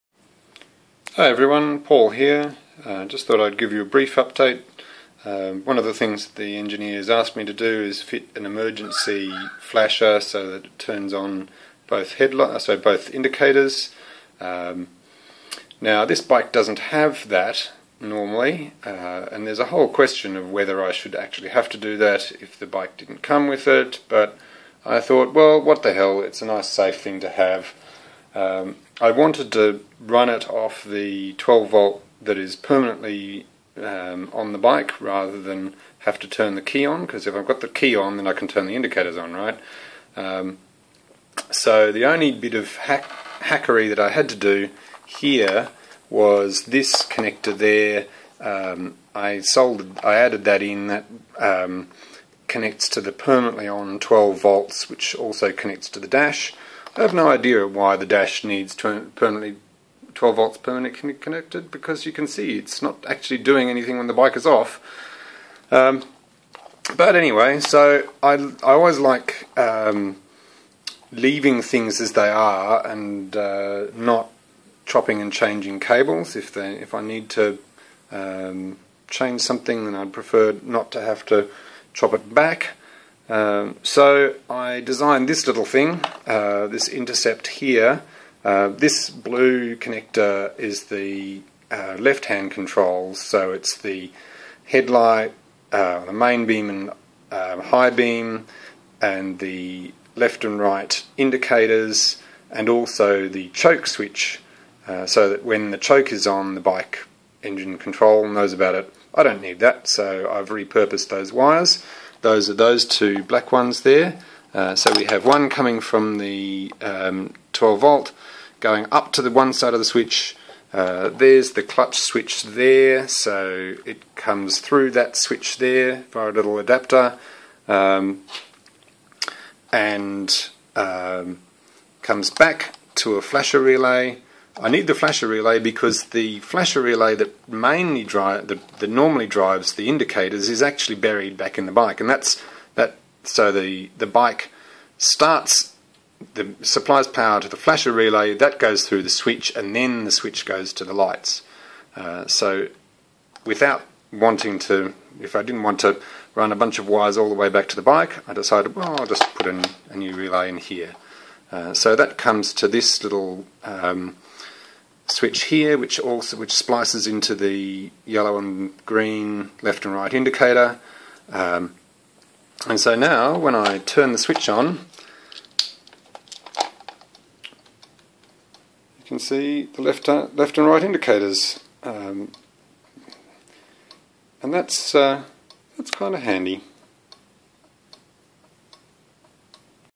Adding emergency indicators.
The engineer asked for hazard lights to be fitted to the bike.  So I made them.